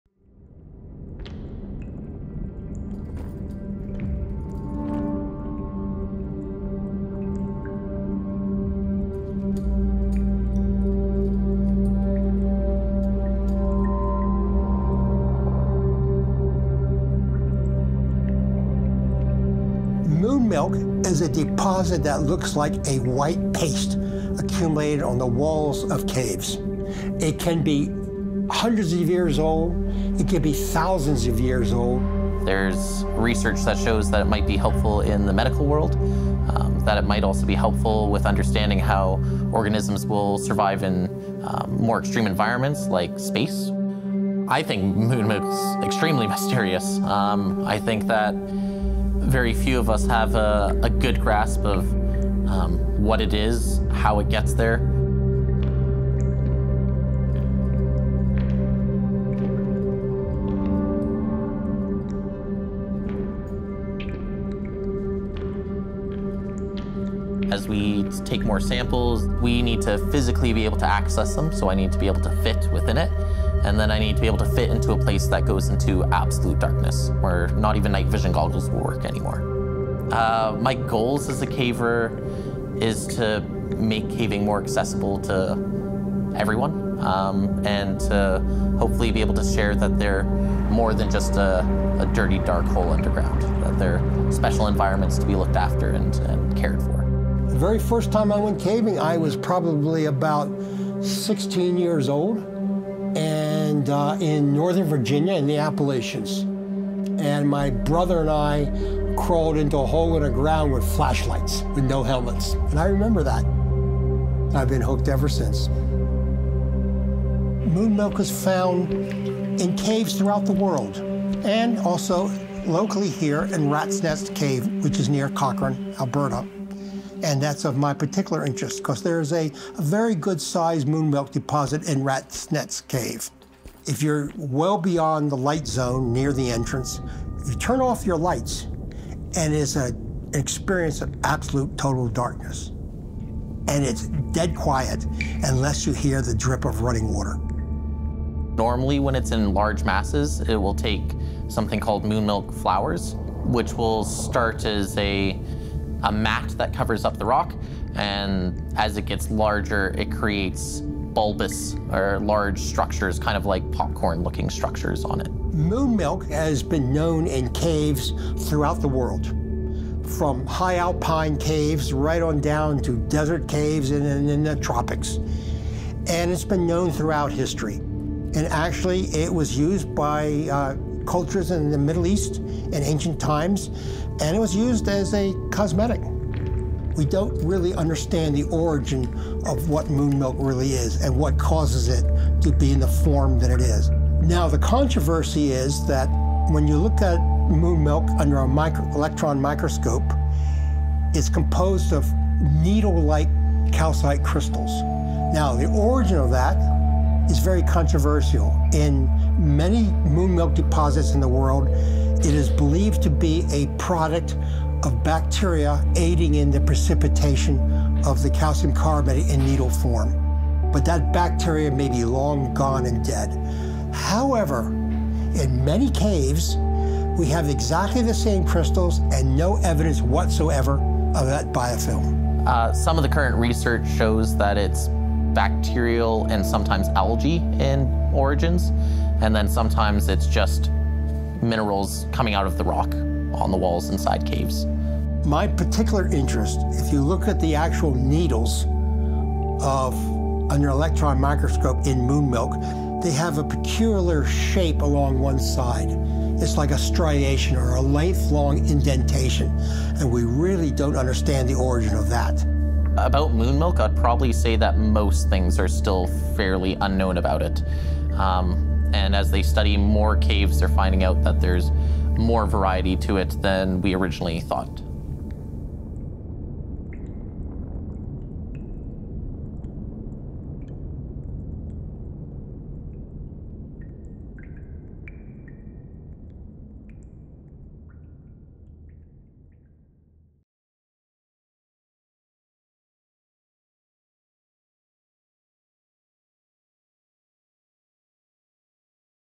Exploring Rat’s Nest cave for moonmilk, a soft popcorn-like substance with mysterious origins
Moonmilk is a mysterious white paste found on the walls inside caves. It’s found in caves across the world, including in Rat’s Nest cave near Canmore, Alberta. Despite its prevalence inside caves, the origins and formation of moonmilk remain largely unknown and hotly debated.

In this CBC Creator Network documentary, expert caver Brent Arnold and geologist and mineralogist Paul Broughton explore the fascinating discovery, collection and potential applications of moonmilk.

Broughton explains how moonmilk could hold the key to certain advancements in medicine, innovative industrial applications and insights into how life can proliferate in hostile environments.

Arnold navigates the darkness in Rat's Nest Cave to collect the mysterious substance while Broughton explains moonmilk’s ancient uses and its diverse formations across the globe.

Credits:
Andrew Koller - Director, Producer, Editor
Ty Ferguson - Cinematography, Colour
Tomas Andel - Sound Design, Mixing

Produced for CBC Creator Network.